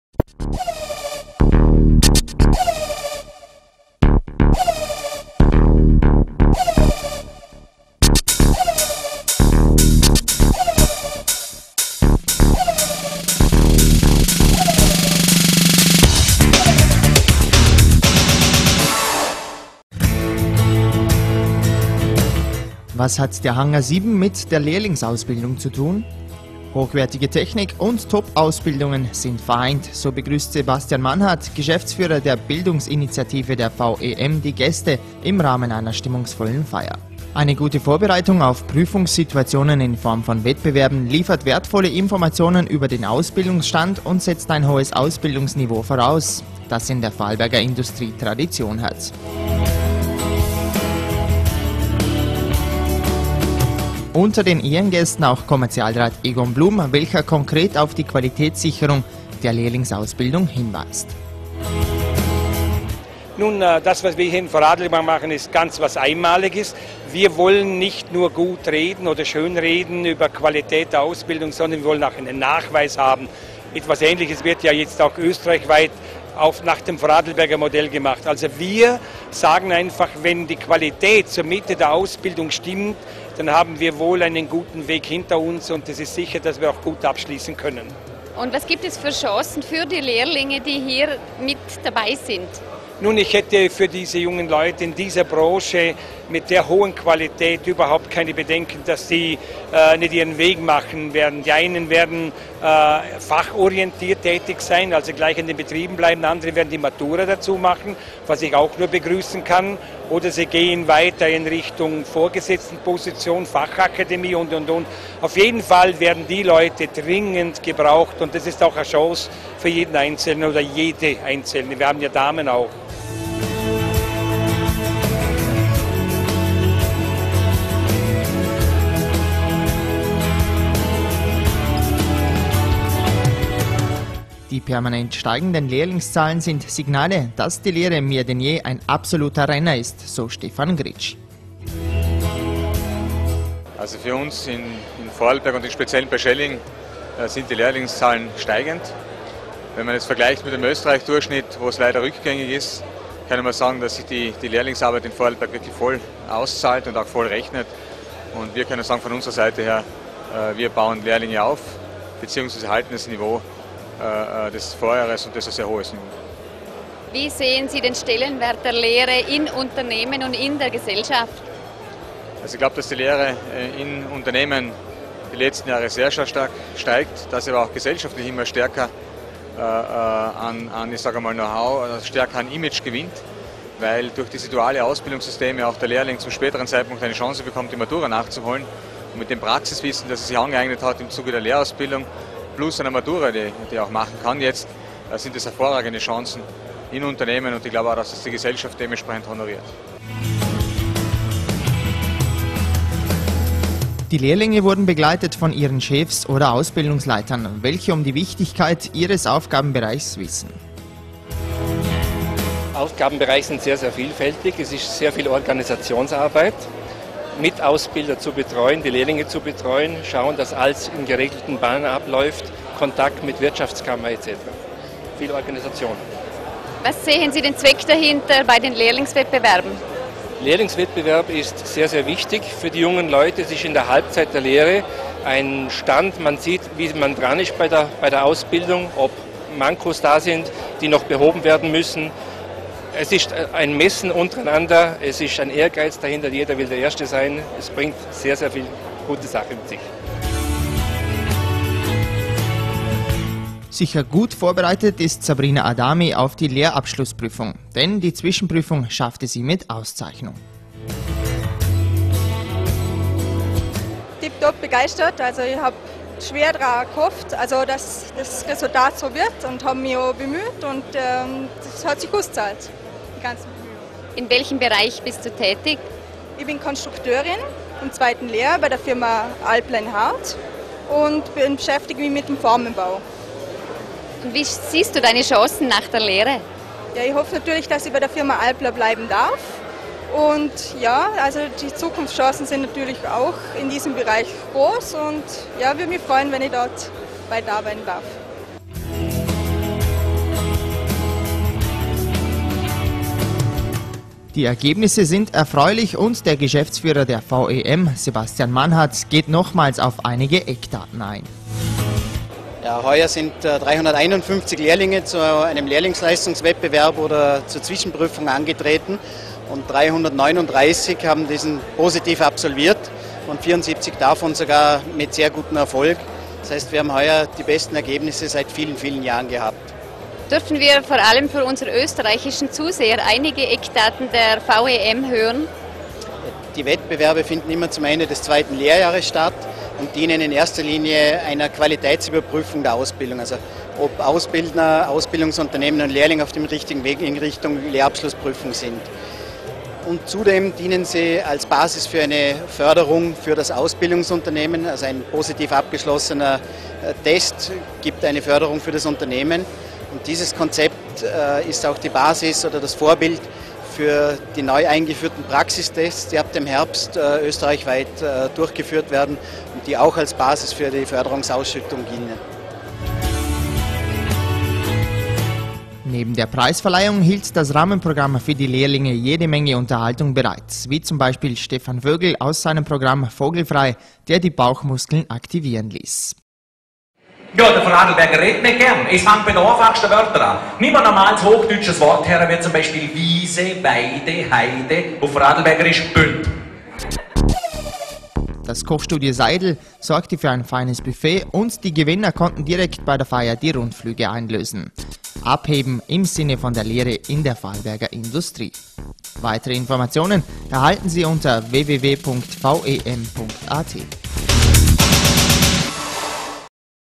News aus der V.E.M.: Lehrlingsleistungswettbewerb 2009 Abschlussfeier
Anerkennung für gute Leistungen wird in der V.E.M., Vorarlberger Elektro- und Metallindustrie hoch geschrieben. Im Rahmen der Abschlussfeier im Hangar 7 in Hohenems wurden die Lehrlinge für Ihr Können in den Zwischenprüfungen ausgezeichnet. Produced by Ländle TV.